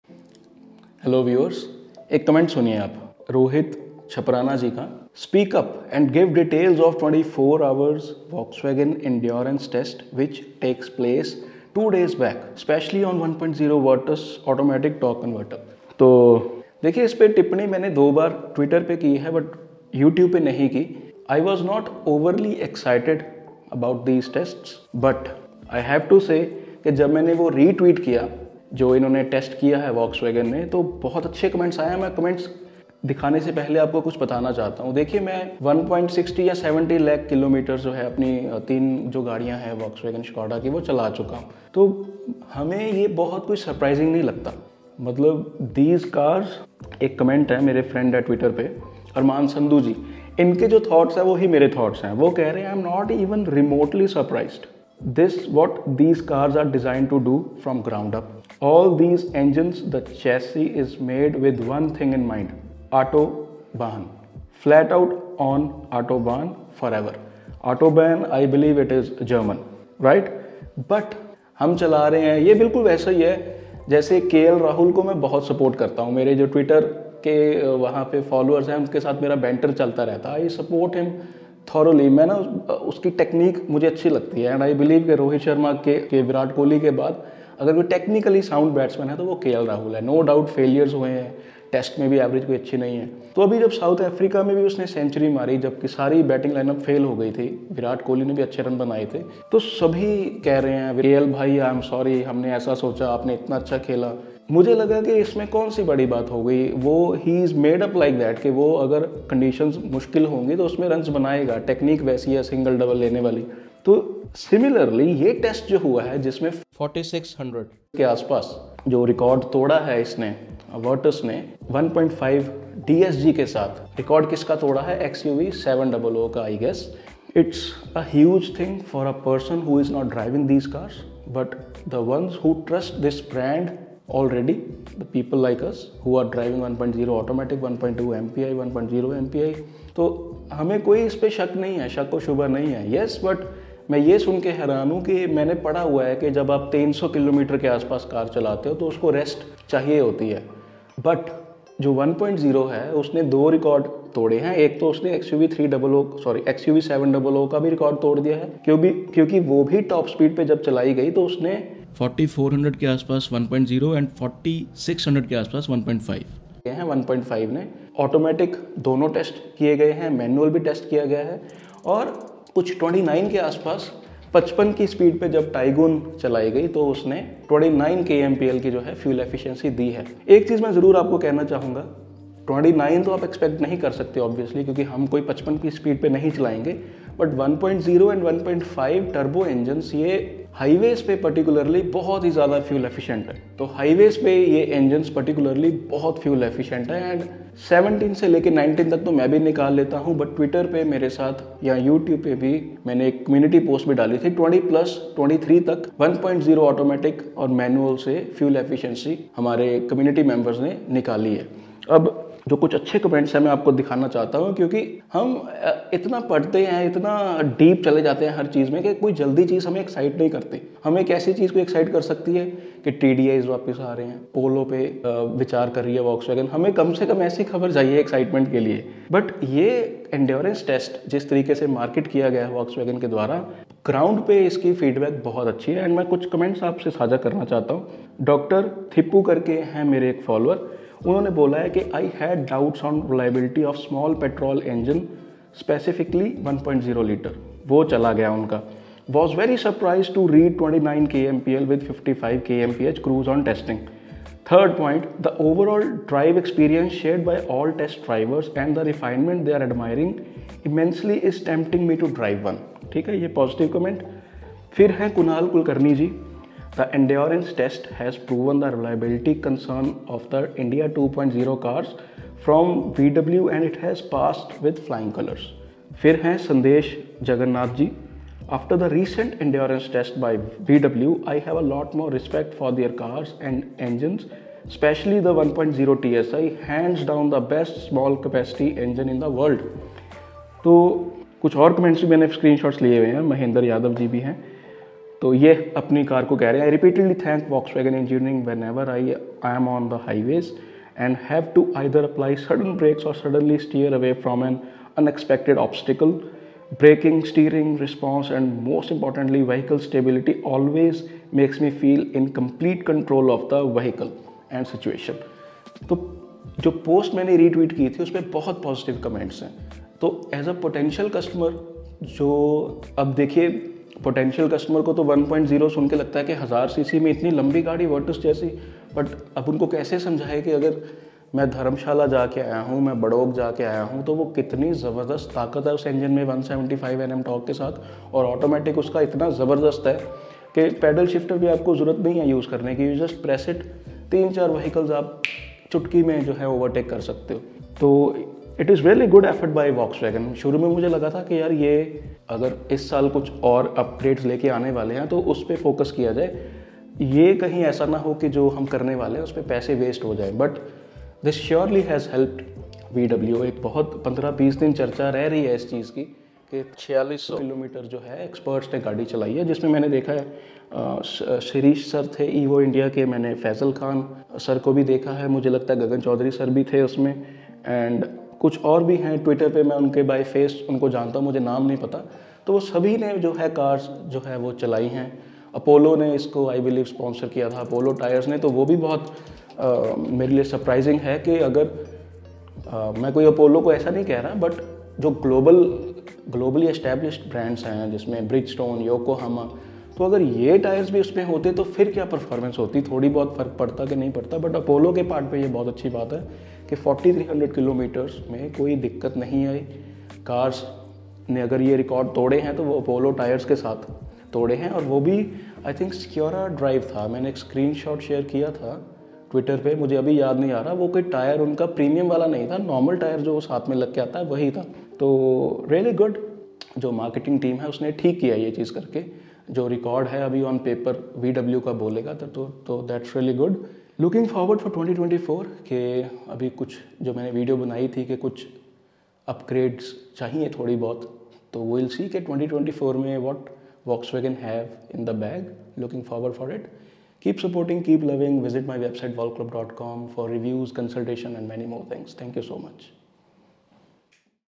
Stylish and Unstoppable: VW Virtus 24-Hour, 4600 Km ENDURANCE Expedition!
Unprecedented Feat: Evo India's Triumph in Sedan Endurance as Volkswagen Virtus 1.5 GT Conquers 24-Hour Marathon at NATRAX, Pithampur, Madhya Pradesh

On February 17, 2024, at 2:00 p.m., Evo India embarked on an extraordinary journey at NATRAX in Pithampur, Madhya Pradesh, aiming to set a new standard for sedan endurance. Behind the wheel of the robust Volkswagen Virtus 1.5 GT, the team delved into an intense 24-hour expedition, pushing the limits of both man and machine.

The clock kept ticking until February 18, 2024, at 2:00 p.m., when Evo India concluded their remarkable journey after covering an astounding 4654.48 kilometres. This outstanding achievement secured the record for the 'maximum distance covered in 24 hours by a sedan.' Throughout the marathon, the Virtus 1.5 GT showcased its reliability, with pit stops only for essential driver changes and refuelling.

Join us in reliving the triumph, as Evo India, with sheer determination and the Volkswagen Virtus 1.5 GT's exceptional performance, etched a new chapter in the history of sedan endurance on February 18, 2024.

Phone Used (iPhone 13 Pro)
Sony a7C
Rode Wireless Go 2 Mic
Hand Tripod (Manfrotto)